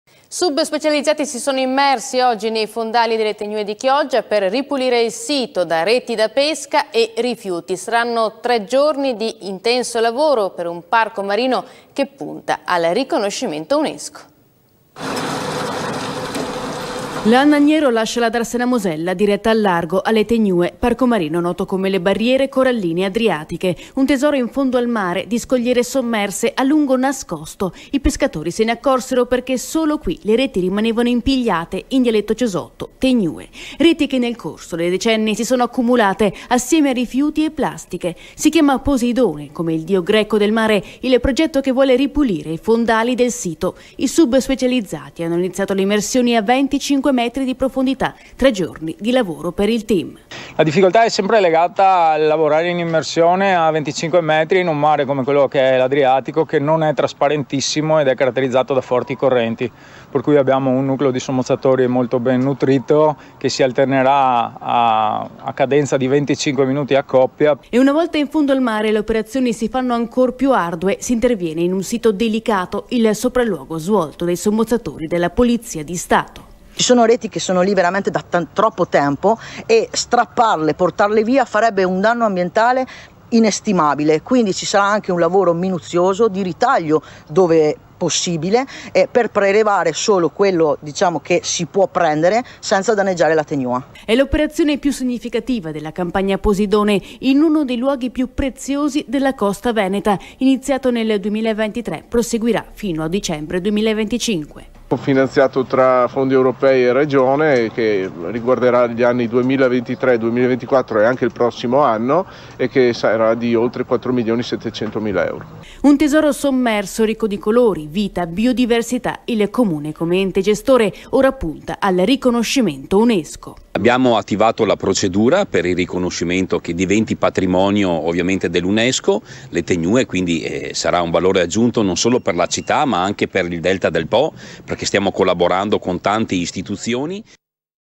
Sub si sono immersi nei fondali delle tegnue di Chioggia per ripulire il sito
Sub specializzati si sono immersi nei fondali delle tegnue di Chioggia per ripulire il sito da reti da pesca e rifiuti. L'operazione, parte del progetto "Posidone", durerà tre giorni e mira a proteggere questo prezioso parco marino, noto come le barriere coralline adriatiche, che ambisce al riconoscimento UNESCO. Le tegnue, scogliere sommerse ricche di biodiversità, sono state danneggiate da anni di accumulo di reti e plastica. I sommozzatori, lavorando a 25 metri di profondità in condizioni difficili, svolgeranno un'operazione delicata per rimuovere i rifiuti senza danneggiare l'ecosistema.